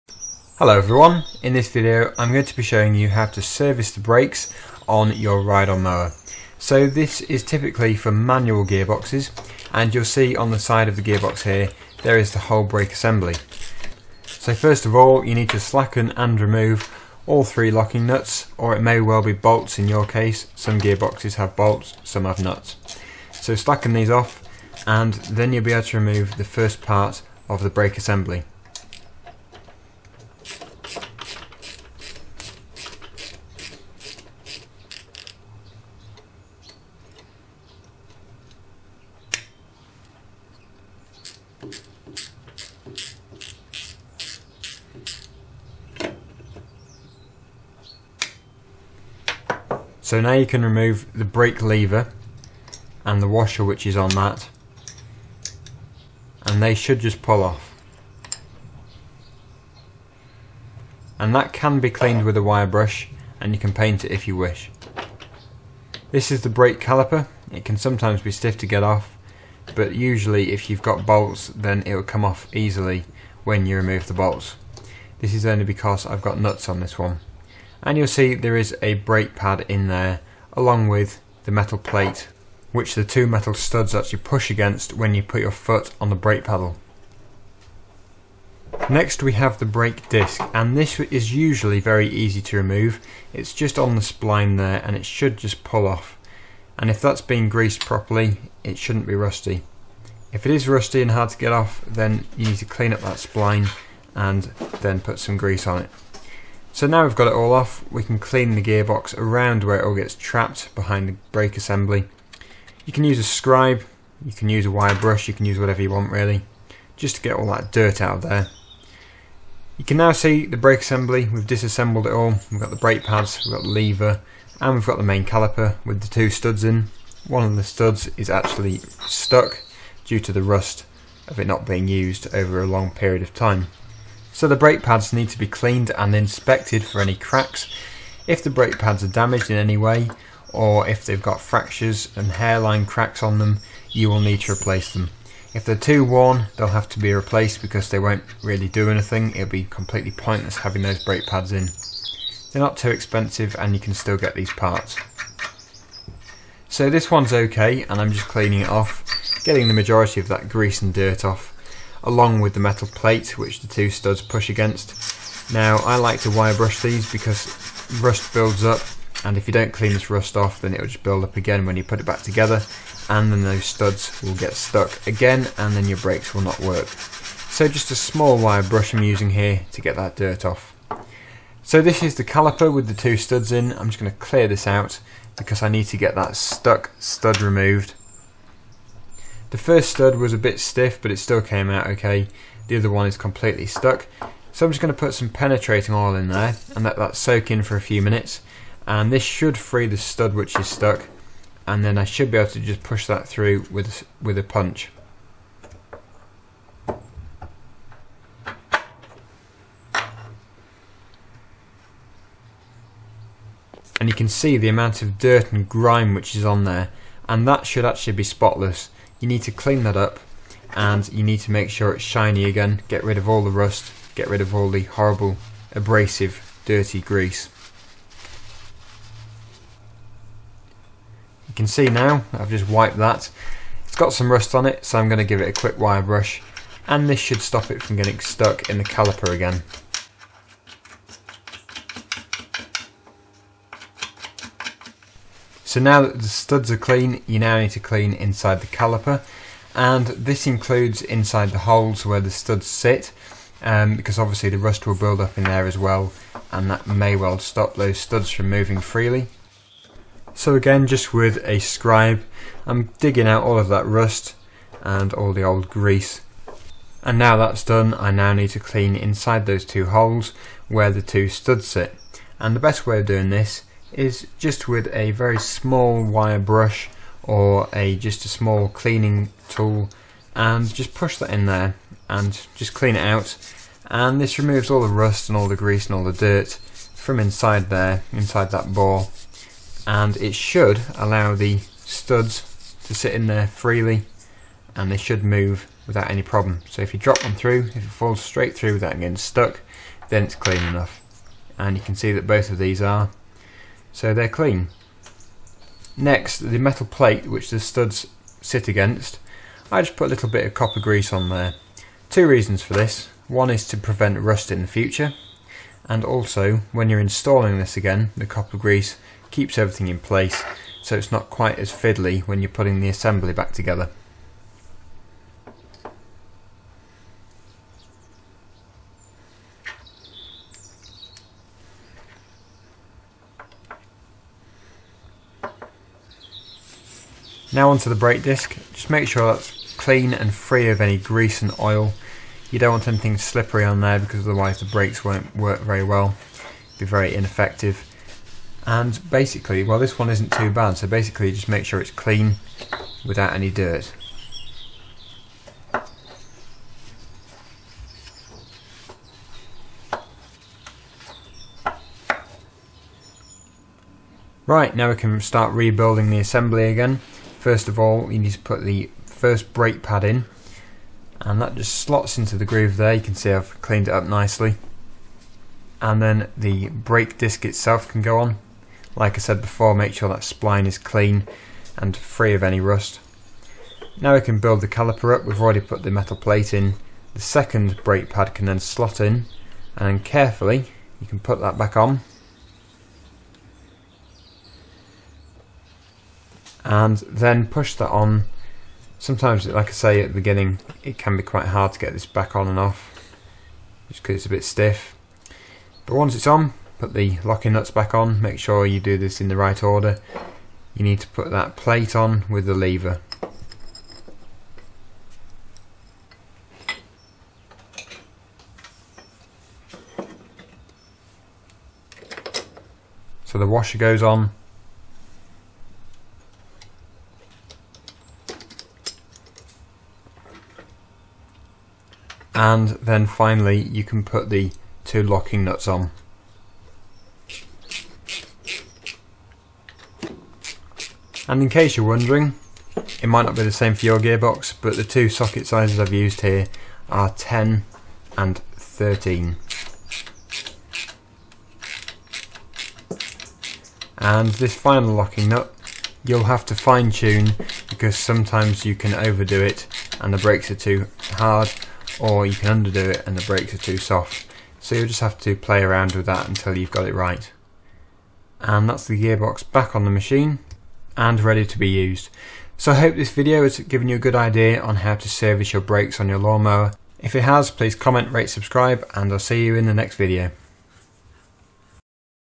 How to: Service Lawnmower Brakes
In this video I show you how to service, maintain and clean the brakes in a ride on mower / lawn tractor.